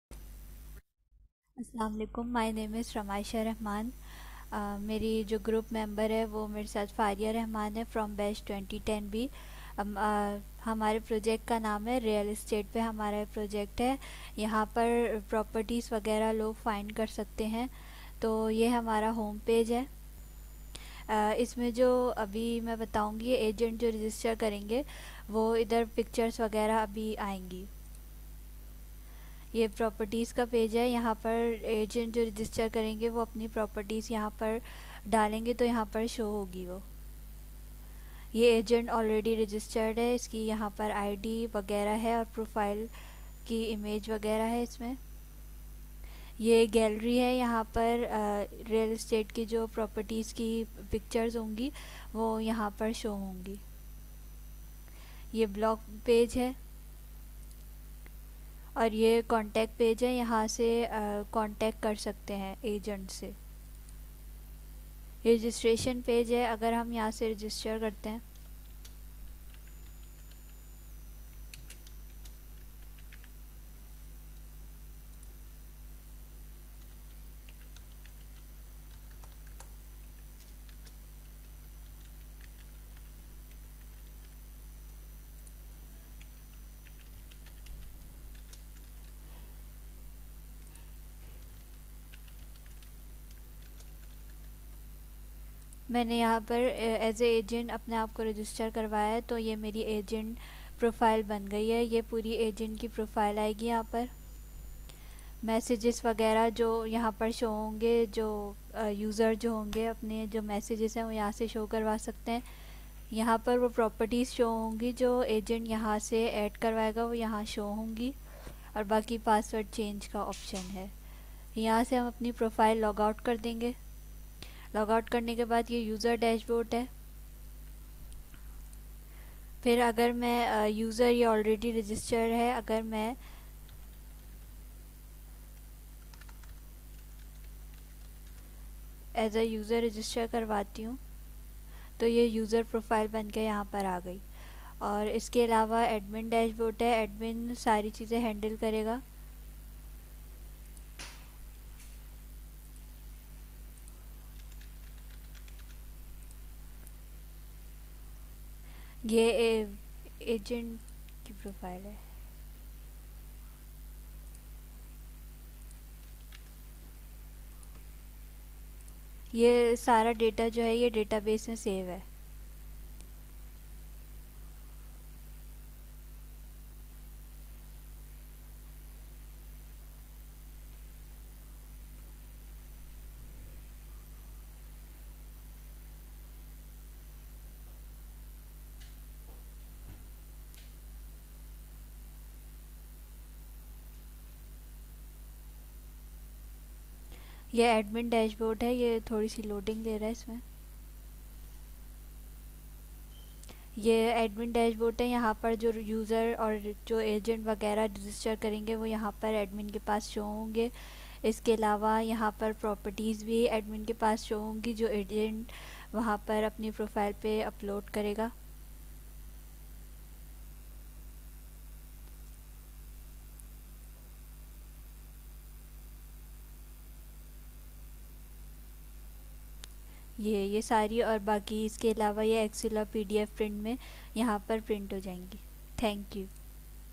REAL STATE
Project Title : REAL STATE
Team Members : Ms. FARIA  REHMAN
Ms. RAMAISHA  REHMAN

Project Description :
A real estate website where agent create an account and add or delete property in other hand buyer create an account and find property which agent added. All these   will be handle by admin